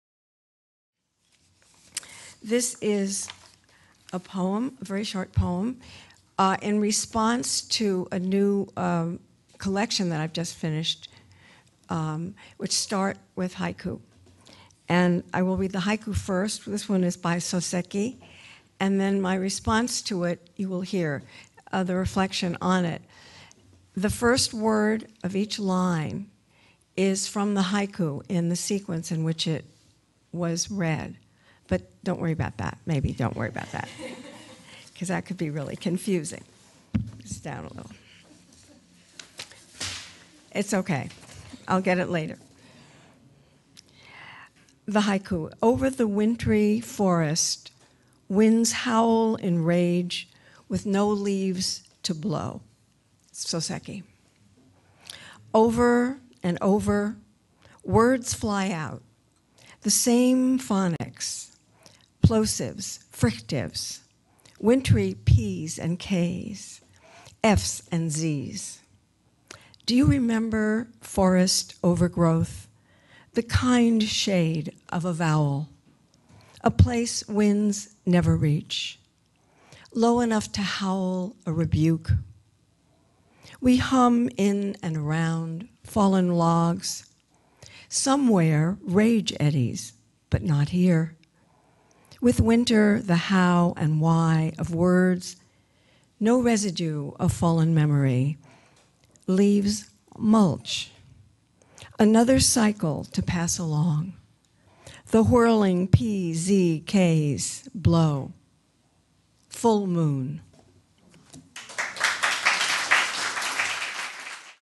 Judith Pacht reads her poem, a response to Haiku by Natsume Soseki
Judith Pacht reads her poem, a response to  Haiku by Natsume Soseki, at the Askew poetry journal reading at Beyond Baroque on December 7, 2013.  This was a celebration to mark the publication of Askew #15.  Beyond Baroque is in Venice, California. Video by Phil Taggart